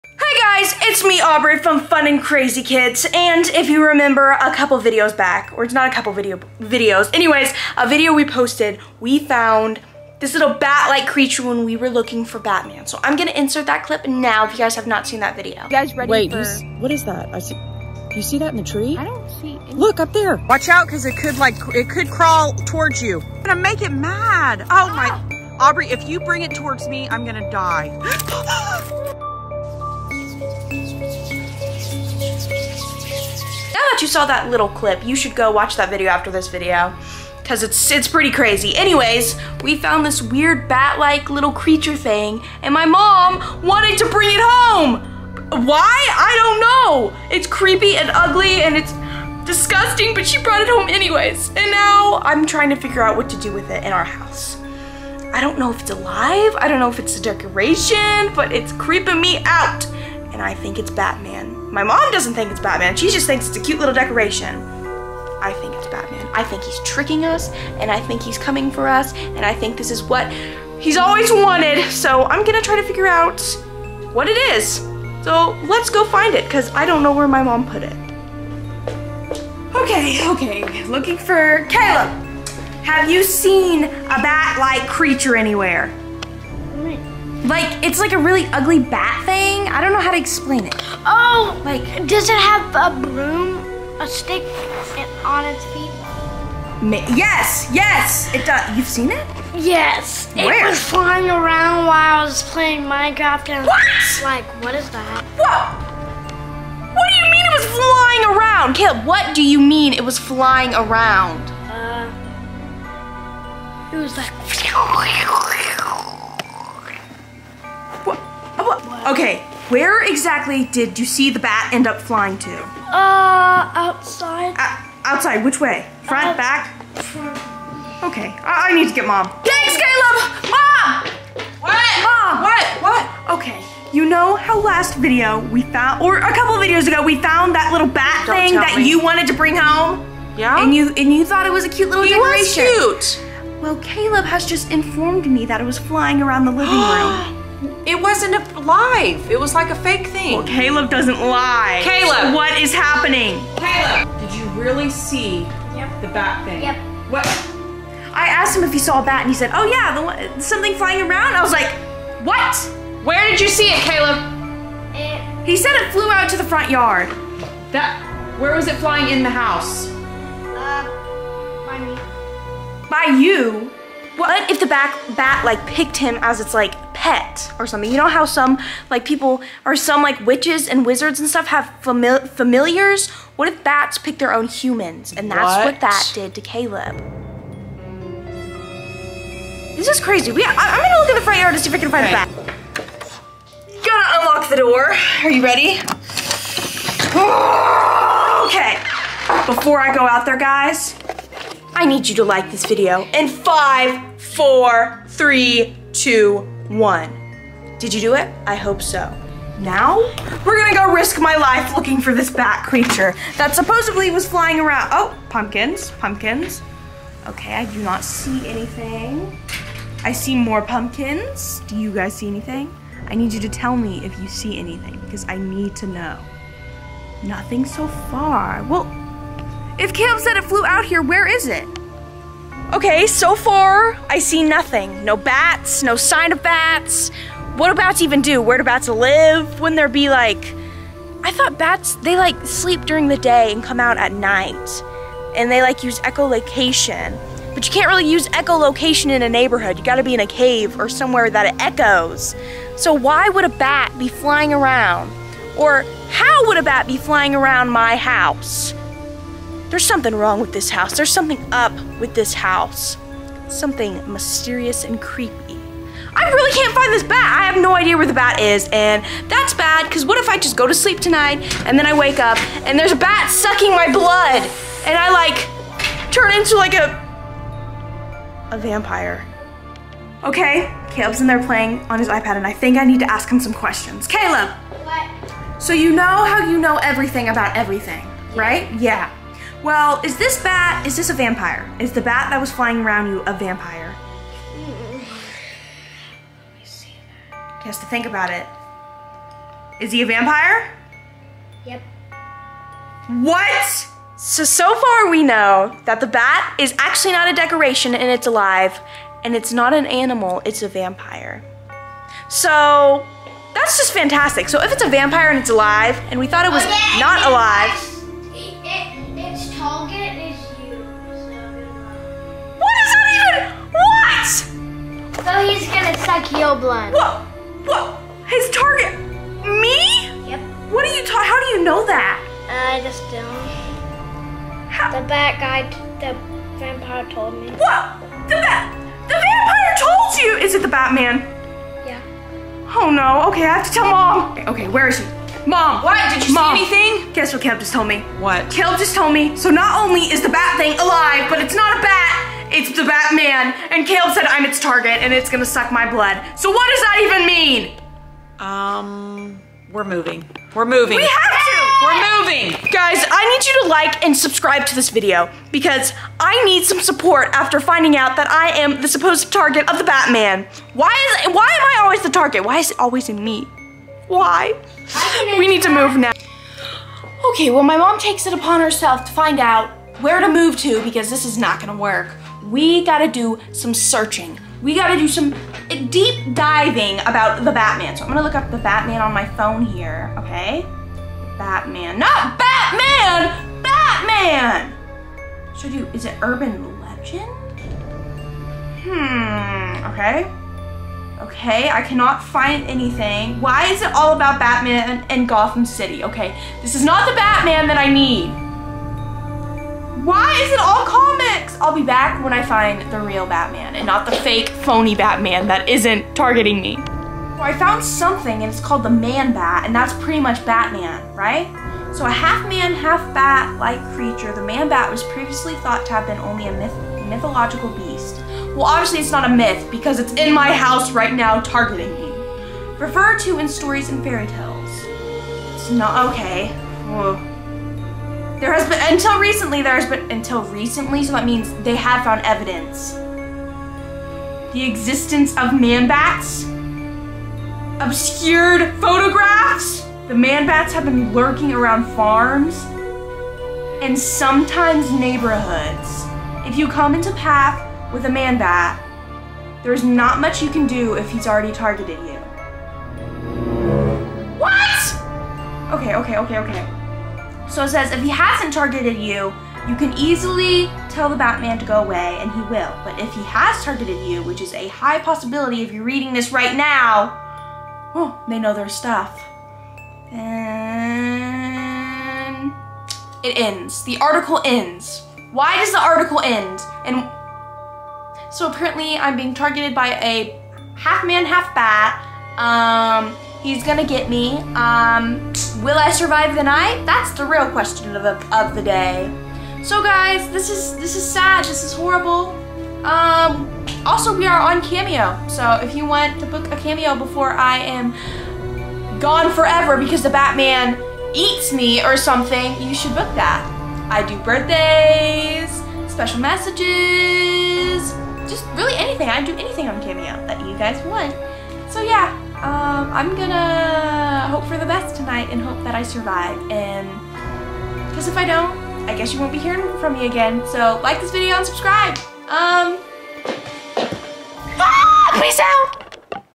BAT MAN CREATURE in the HOUSE?! THE LEGEND of BAT-MAN URBAN leGEND (SCARY)
*This video might be a little scary for our younger viewers.

Have you ever heard of the legend of the bat man aka the man that is half bat and half man? No I am not talking about the super hero. This batman is a creature that has the body of a man and the head of a bat. Well last week Aubrey and her mom Mellisa went for a hike and found this weird bat creature. Mom decided to bring it home and now weird stuff keeps happening. Legend has it that the bat man will shine a red light and make the victims freeze so they cant' see him.Hopefully it's just a scary story What do you guys think?